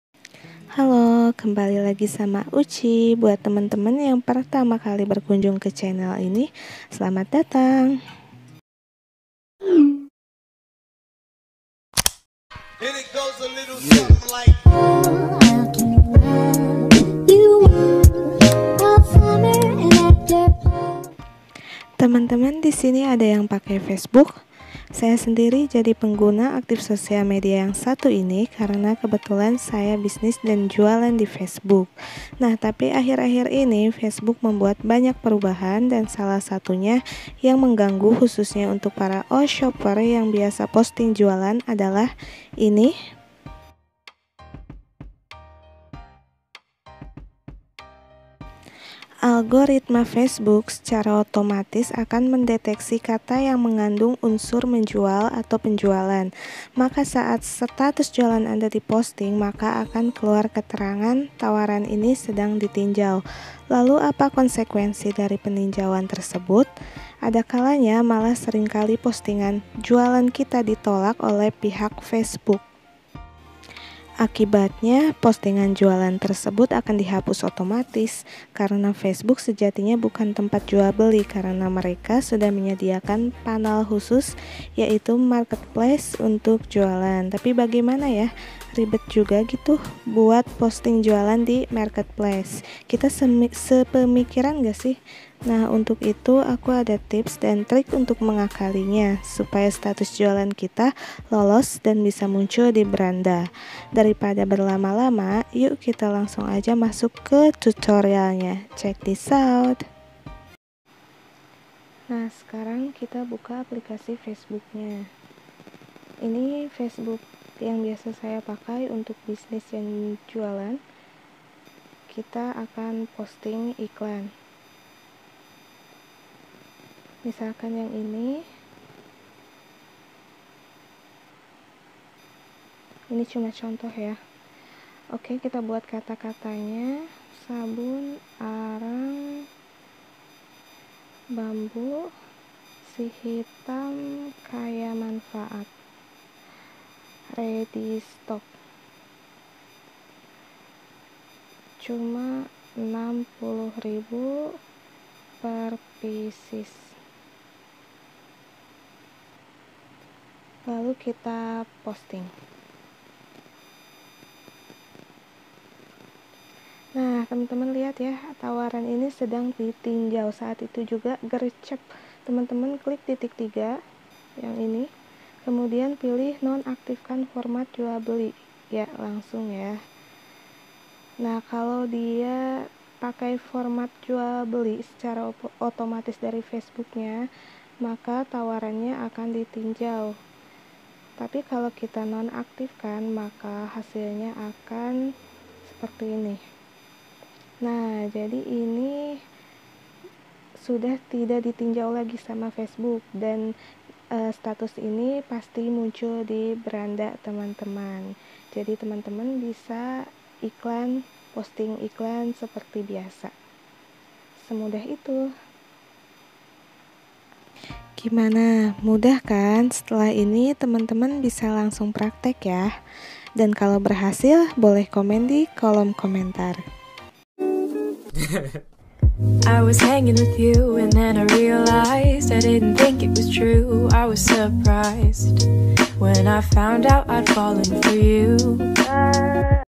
Cara Mengatasi Postingan Facebook di Tinjau dan Gagal Posting
Video ini sharing untuk memudahkan teman² yang sama² sedang berjuang mencari rezeki dengan berjualan di facebook. Hehe..

Silakan teman² boleh mampir ke facebook saya untuk tahu bisnis yang sedang saya jalani.

Terimakasih